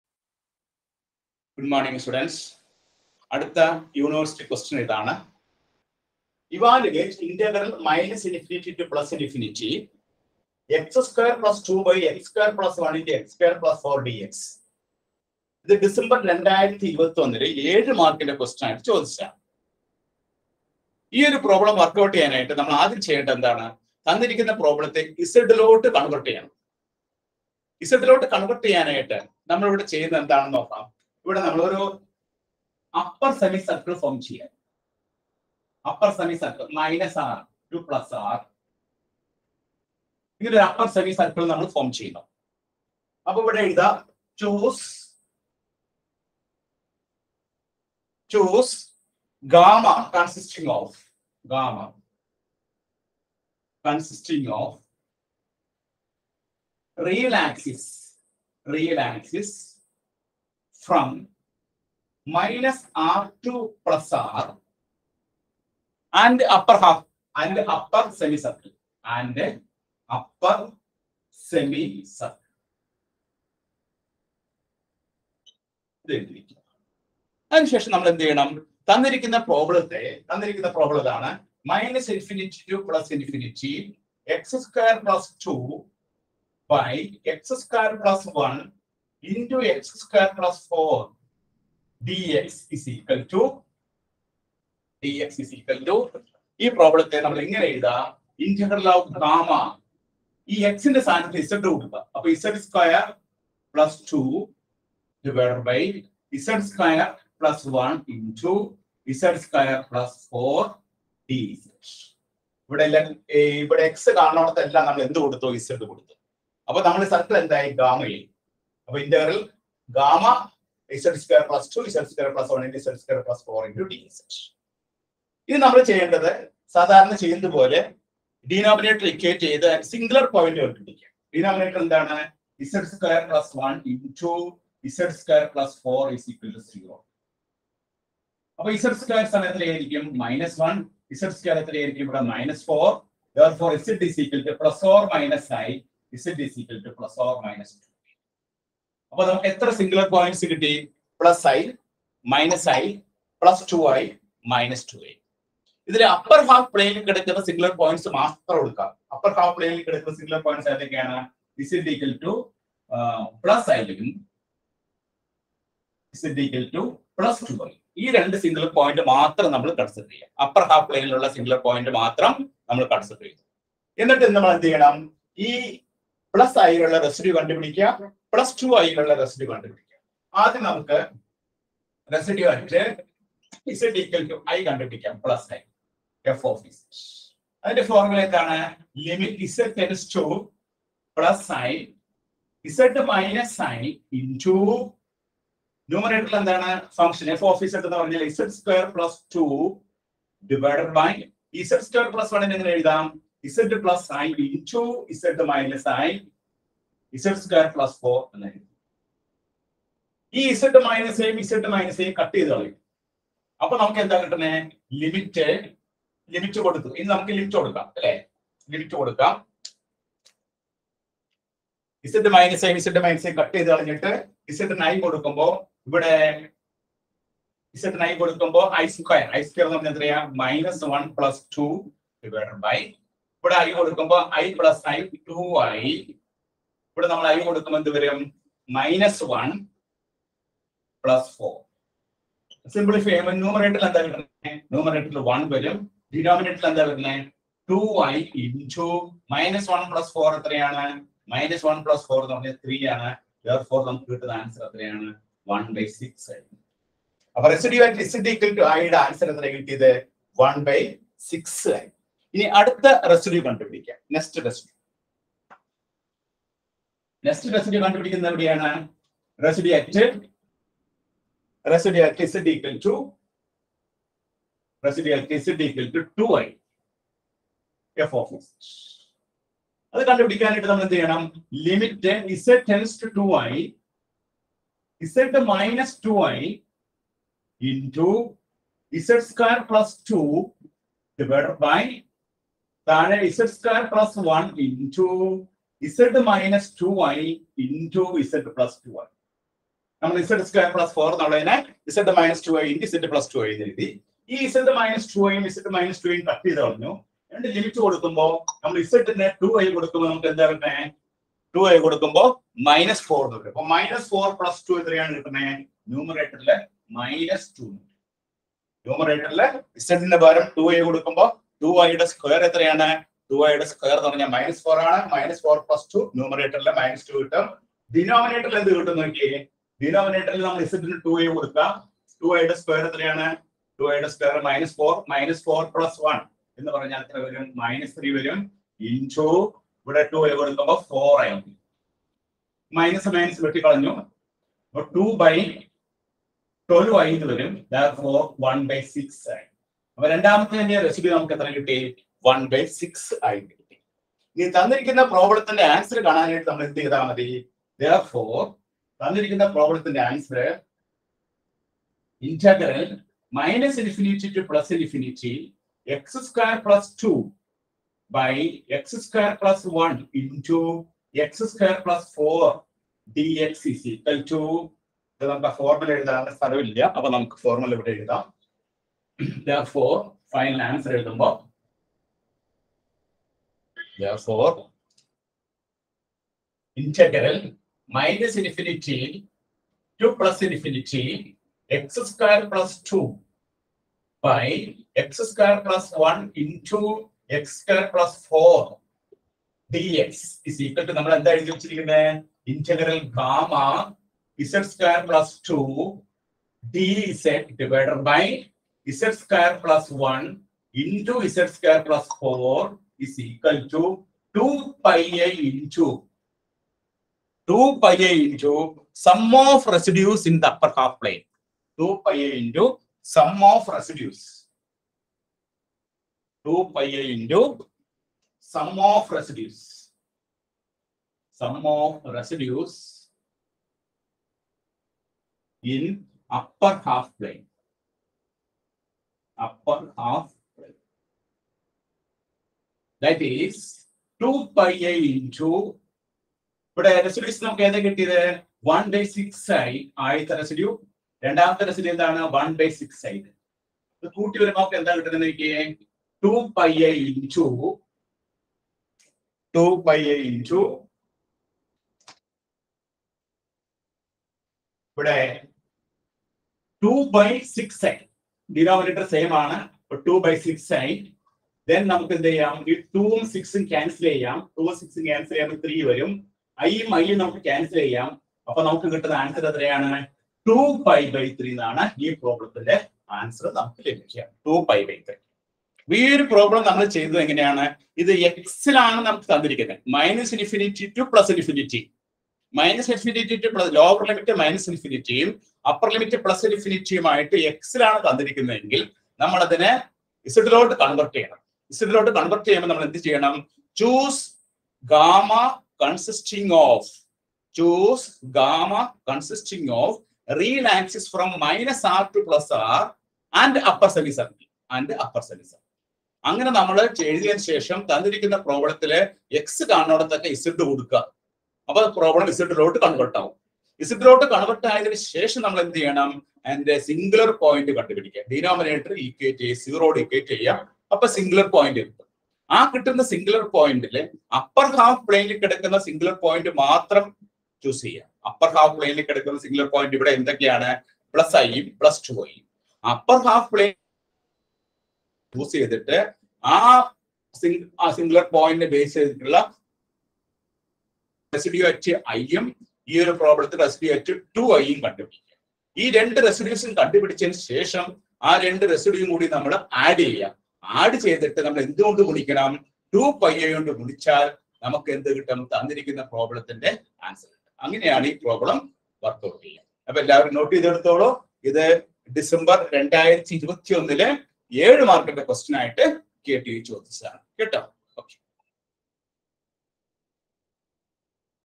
UNIVERSITY QUESTION-17 I MODULE-5 I Complex Variable-Residue Integration | MAT201|KTU-2019 I SEM-S3
This playlist contains University questions -Laurent’s series(without proof ), zeros of analytic functions, singularities, poles, removable singularities, essential singularities, Residues, Cauchy Residue theorem (without proof), Evaluation of definite integral using residue theorem, Residue integration of real integrals – integrals of rational functions of  and    , integrals of improper integrals .
 This video will help you understand problems related to  complex integration